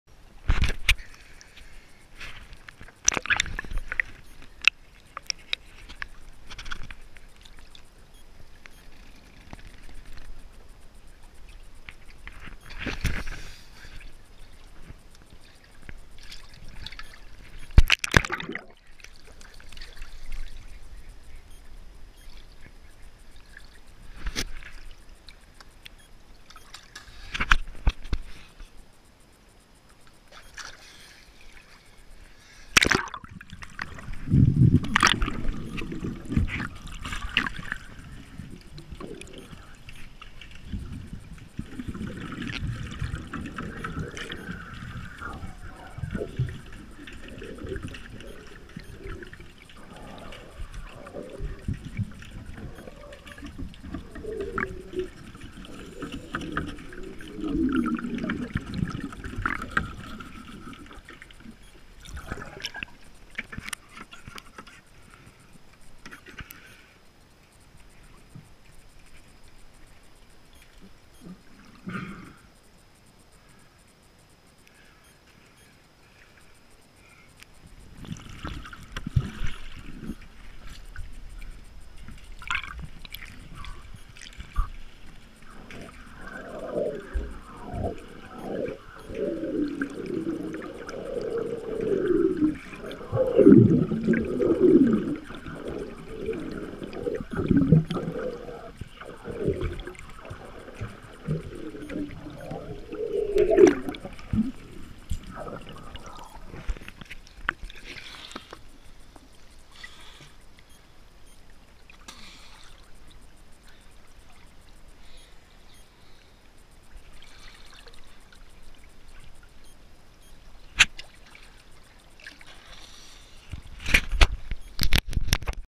Lucky Me 11:20:22
Peninsula Dive Services cleaning for Bruce Nobles boat cleaning.